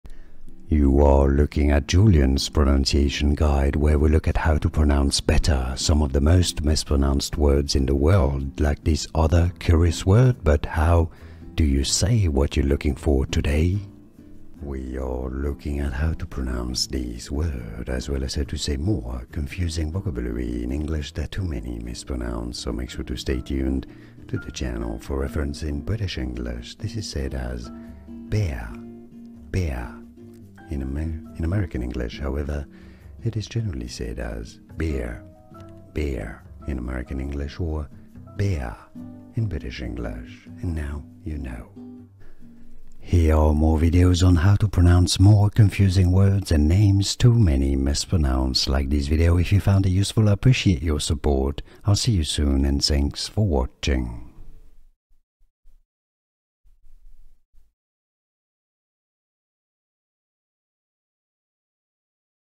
How to Pronounce Bare (correctly!)
Welcome! Here you will hear How to Pronounce Bare (correctly!) i.e. the “correct” pronunciation.

You can skip the intro through the time stamps below:
00:00 - Pronunciation Intro
00:15 - How to Pronounce Bare (correctly!)